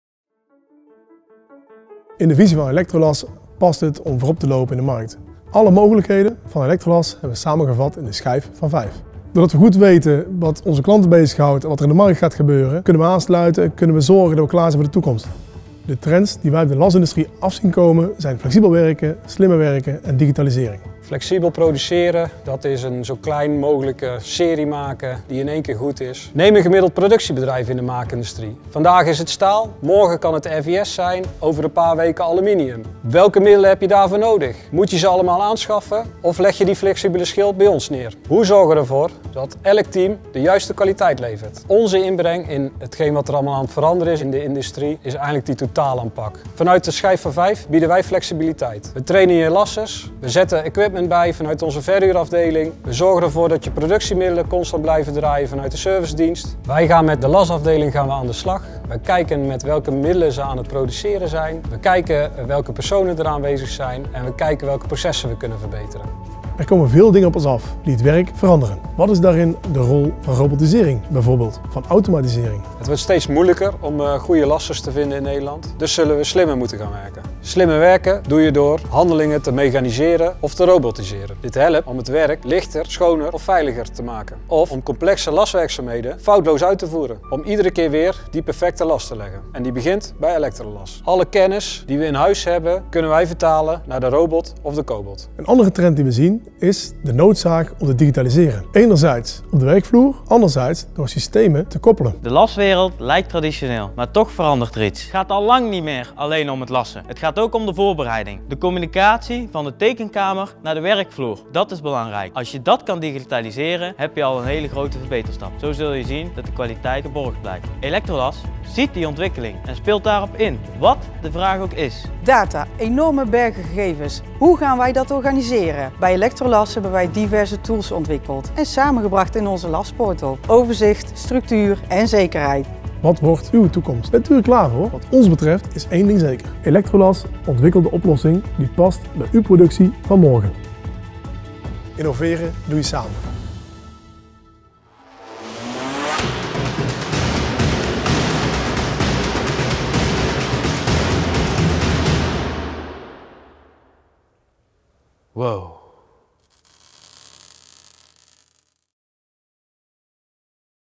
De laswerkplaats van morgen
Onder de titel 'De laswerkplaats van morgen' vallen verschillende toekomst ontwikkelingen in de lasindustrie. De zogenaamde trends die ook genoemd worden in Smart Industry of in Duitsland bekend onder Industrie 4.0.

Met de laswerkplaats van morgen richt Elektrolas zich op het ondersteunen van de maakindustrie om te innoveren door te digitaliseren, slimmer te werken en flexibeler te werken.

Hoe bereid je je voor op de productietrends van morgen? Welke rol is weggelegd voor lassen en lassers? Elektrolas ontwikkelt dé oplossing die past bij jouw productie van morgen. Innoveren doe je samen.